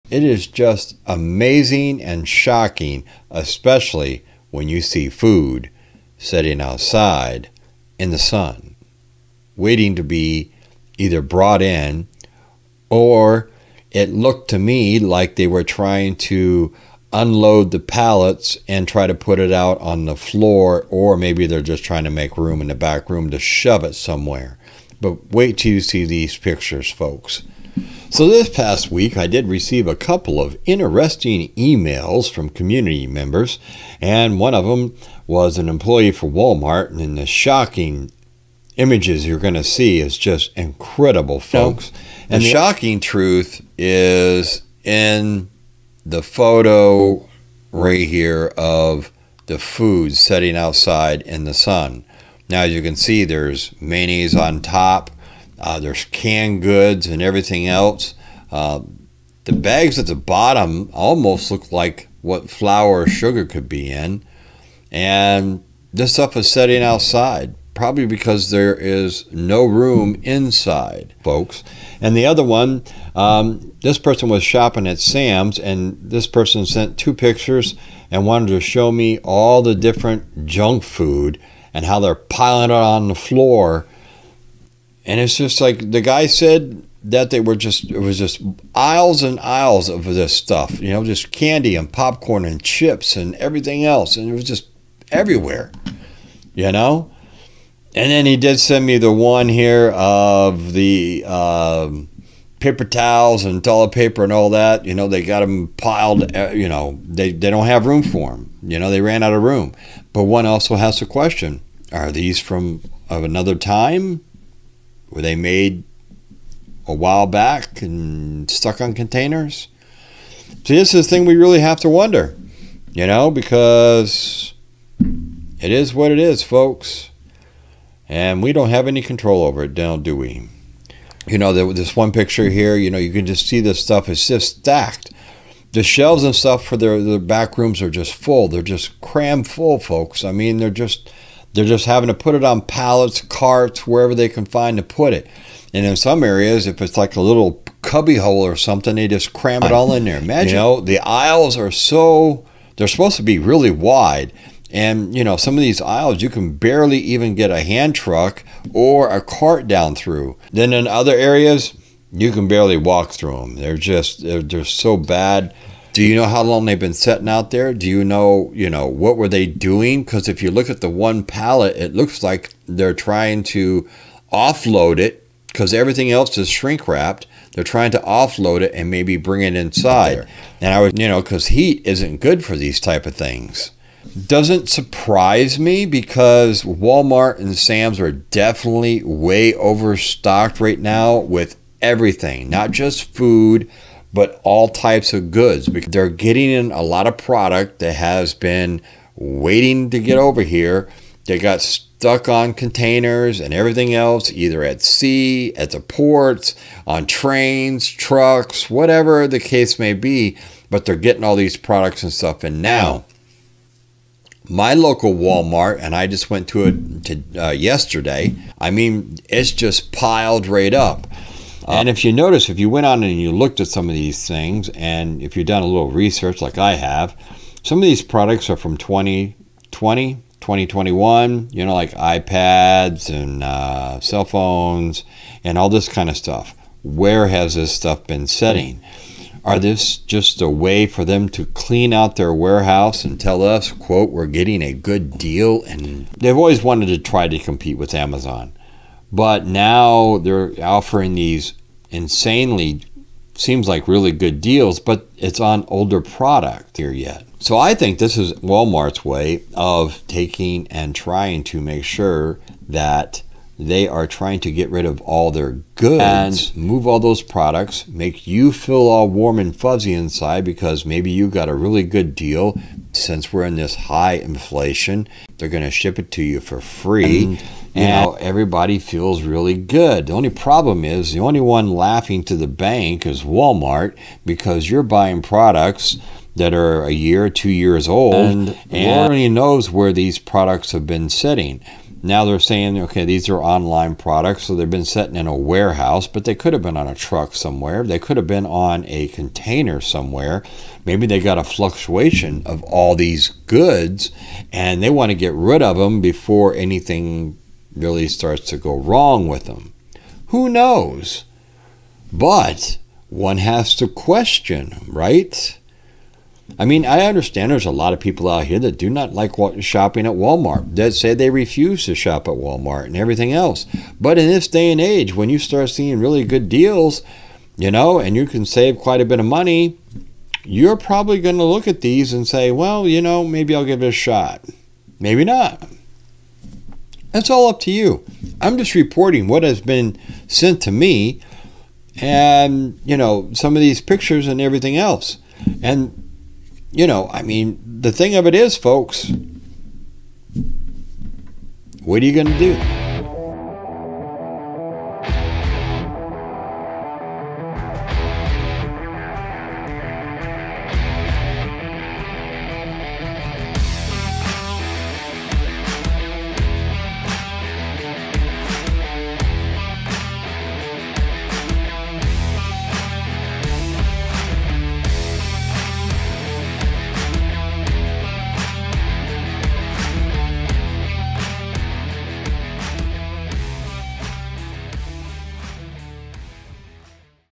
Walmart is Over Stocked Again Here's Why!
Walmart is Over Stocked Again Here's Why! With all the stores they are closing where does all that product go?? Well I found out.